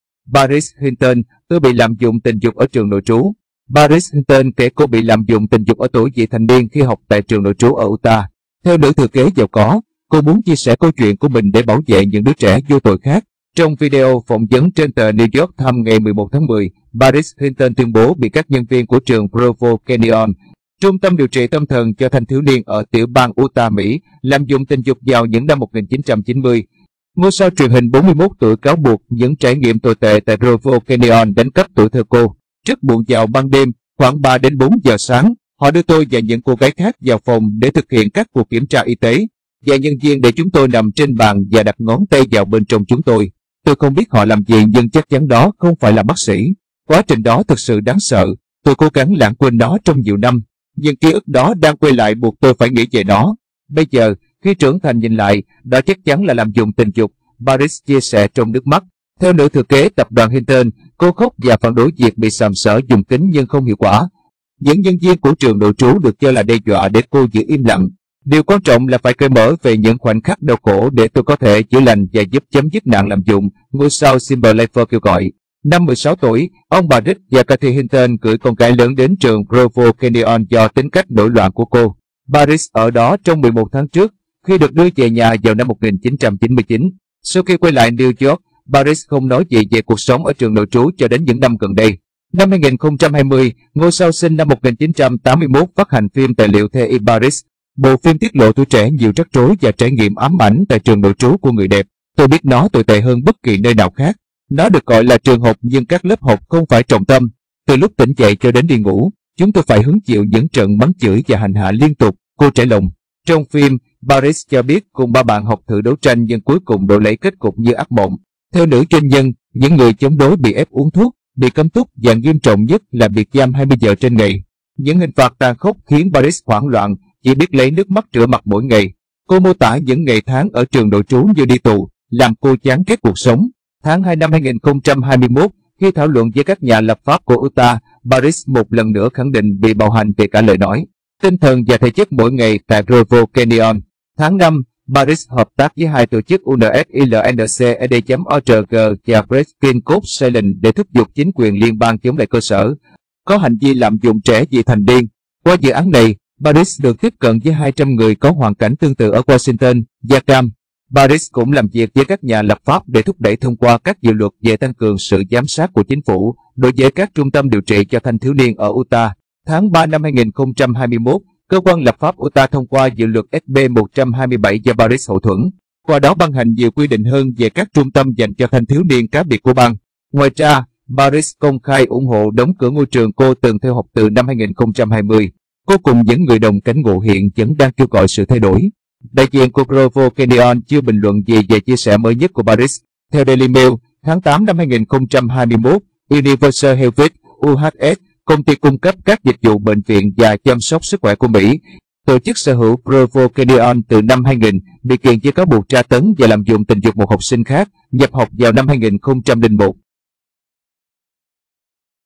Paris Hilton: 'Tôi bị lạm dụng tình dục ở trường nội trú'
Paris Hilton kể cô bị lạm dụng tình dục ở tuổi vị thành niên khi học tại trường nội trú ở Utah. Theo nữ thừa kế giàu có, cô muốn chia sẻ câu chuyện của mình để bảo vệ những đứa trẻ vô tội khác.Nữ thừa kế 41 tuổi tích cực tham gia hoạt động ngăn chặn lạm dụng trẻ em trong trường hợp. Ảnh: Getty Images.
Trong video phỏng vấn trên tờ New York Times ngày 11/10, Paris Hilton tuyên bố bị các nhân viên của trường Provo Canyon, trung tâm điều trị tâm thần cho thanh thiếu niên ở tiểu bang Utah (Mỹ), lạm dụng tình dục vào những năm 1990. Ngôi sao truyền hình 41 tuổi cáo buộc những trải nghiệm tồi tệ tại Provo Canyon “đánh cắp” tuổi thơ cô.
Trong phim, Paris cho biết cùng ba bạn học thử đấu tranh nhưng cuối cùng đổi lấy kết cục như ác mộng. Theo nữ doanh nhân, những người chống đối bị ép uống thuốc, bị cấm túc và nghiêm trọng nhất là biệt giam 20 giờ/ngày. Những hình phạt tàn khốc khiến Paris hoảng loạn, chỉ biết lấy nước mắt “rửa” mặt mỗi ngày. Cô mô tả những ngày tháng ở trường nội trú như đi tù, làm cô chán ghét cuộc sống.
Paris cũng làm việc với các nhà lập pháp để thúc đẩy thông qua các dự luật về tăng cường sự giám sát của chính phủ đối với các trung tâm điều trị cho thanh thiếu niên ở Utah. Tháng 3/2021, Cơ quan lập pháp Utah thông qua dự luật SB127 do Paris hậu thuẫn, qua đó ban hành nhiều quy định hơn về các trung tâm dành cho thanh thiếu niên cá biệt của bang.
Tháng 5, Paris hợp tác với hai tổ chức Unsilenced.org và Breaking Code Silence để thúc giục chính quyền liên bang chống lại cơ sở có hành vi lạm dụng trẻ vị thành niên. Qua dự án này, Paris được tiếp cận với 200 người có hoàn cảnh tương tự ở Washington, DC.
Ngoài ra, Paris công khai ủng hộ đóng cửa ngôi trường cô từng theo học từ năm 2020. Cô cùng những người đồng cảnh ngộ hiện vẫn đang kêu gọi sự thay đổi.
“Rất muộn vào ban đêm, khoảng 3-4h sáng, họ đưa tôi và những cô gái khác vào phòng để thực hiện các cuộc kiểm tra y tế. Vài nhân viên để chúng tôi nằm trên bàn và đặt ngón tay vào bên trong chúng tôi. Tôi không biết họ làm gì nhưng chắc chắn đó không phải là bác sĩ. Quá trình đó thực sự đáng sợ. Tôi cố gắng lãng quên nó trong nhiều năm. Nhưng ký ức đó đang quay lại buộc tôi phải nghĩ về nó. Bây giờ, khi trưởng thành nhìn lại, đó chắc chắn là lạm dụng tình dục”, Paris chia sẻ trong nước mắt.
Paris biểu tình yêu cầu đóng cửa trường Provo Canyon năm 2020. Ảnh: Instagram.
“Tôi biết nó tồi tệ hơn bất kỳ nơi nào khác. Nó được gọi là trường học nhưng các lớp học không phải trọng tâm. Từ lúc tỉnh dậy cho đến đi ngủ, chúng tôi phải hứng chịu những trận mắng chửi và hành hạ liên tục”, cô trải lòng.
Tháng 2/2021, khi thảo luận với các nhà lập pháp của Utah, Paris một lần nữa khẳng định bị bạo hành về cả lời nói, tinh thần và thể chất mỗi ngày tại Provo Canyon.
“Điều quan trọng là phải cởi mở về những khoảnh khắc đau khổ để tôi có thể chữa lành và giúp chấm dứt nạn lạm dụng”, ngôi sao Simple Life kêu gọi.
Năm 16 tuổi, ông bà Rick và Kathy Hilton gửi con gái lớn đến trường Provo Canyon do tính cách nổi loạn của cô. Paris ở đó trong 11 tháng trước khi được đưa về nhà vào năm 1999. Sau khi quay lại New York, Paris không nói gì về cuộc sống ở trường nội trú cho đến những năm gần đây.
Theo nữ thừa kế tập đoàn Hilton, cô khóc và phản đối việc bị sàm sỡ “vùng kín” nhưng không hiệu quả. Những nhân viên của trường nội trú được cho là đe dọa để cô giữ im lặng.
Đại diện của Provo Canyon chưa bình luận gì về chia sẻ mới nhất của Paris. Theo Daily Mail, tháng 8/2021, Universal Health Services (UHS – công ty cung cấp các dịch vụ bệnh viện và chăm sóc sức khỏe của Mỹ), tổ chức sở hữu Provo Canyon từ năm 2000, bị kiện với cáo buộc tra tấn và lạm dụng tình dục một học sinh khác, nhập học vào năm 2001.
Paris Hilton khóc khi kể lại trải nghiệm đau thương lúc còn thiếu nữ. Ảnh: New York Times.
Năm 2020, ngôi sao sinh năm 1981 phát hành phim tài liệu The Is Paris. Bộ phim tiết lộ tuổi trẻ nhiều rắc rối và trải nghiệm ám ảnh tại trường nội trú của người đẹp.